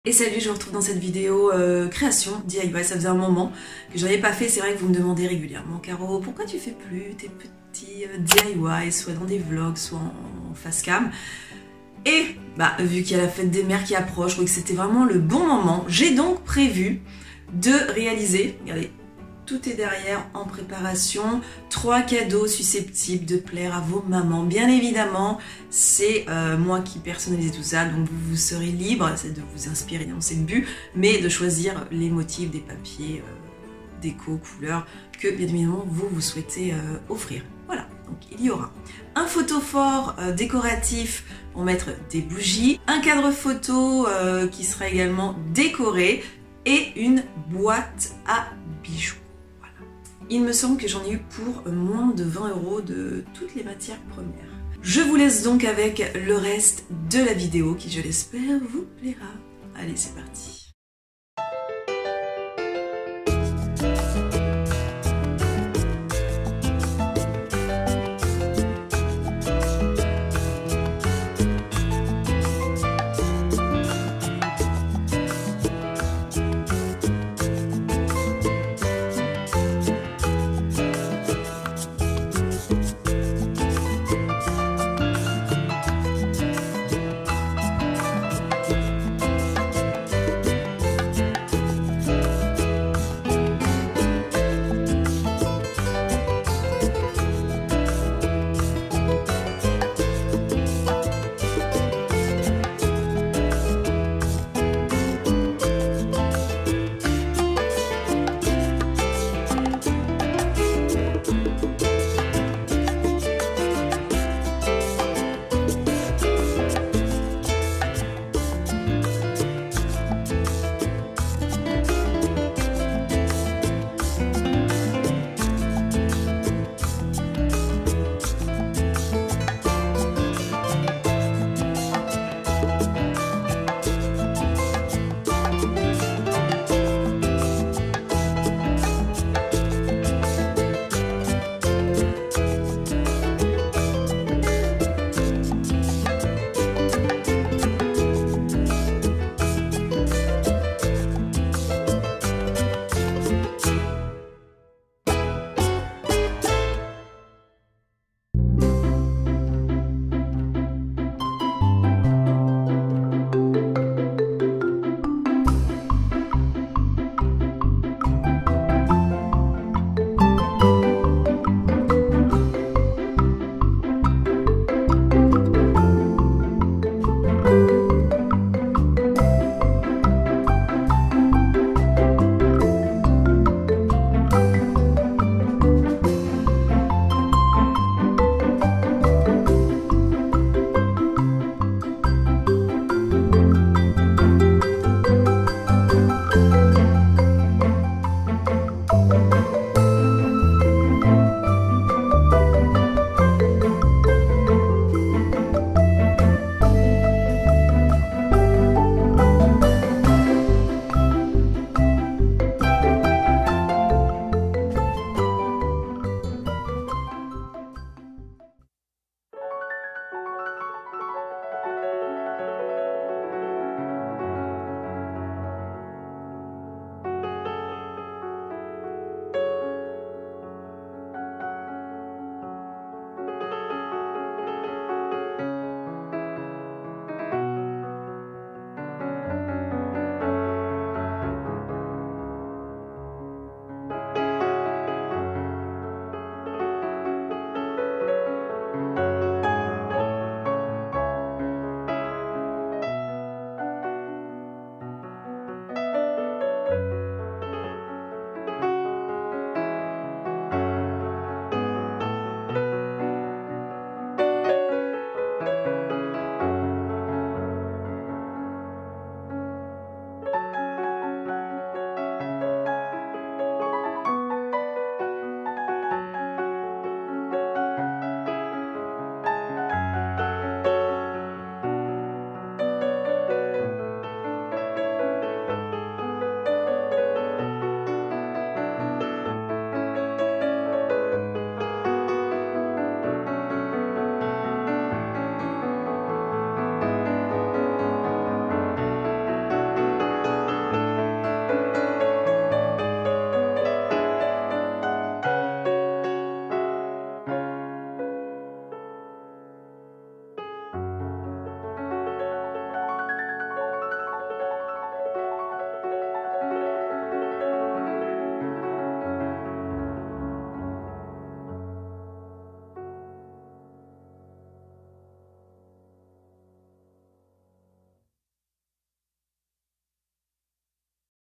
3 DIY FETE DES MERES IDEES CADEAUX FACILES
3 DIY FETE DES MERES IDEES CADEAUX FACILES
Fabrique 3 idées cadeaux faciles à offrir à ta maman pour la fête des mères ! Photophore bougie, cadre photo déco et une boite a bijoux ! il y en a pour tous les goûts !